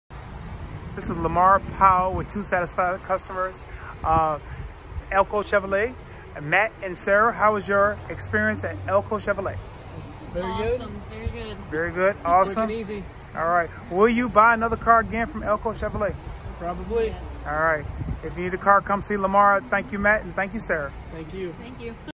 Matt and Sarah's Chevrolet Equinox!
Congratulations to Matt and Sarah on the purchase of their Chevrolet Equinox! With assistance from Lamarr Powell they were able to drive off ELCO's Pre-Owned lot a very happy customer! Enjoy your new ride! ProudNewOwners!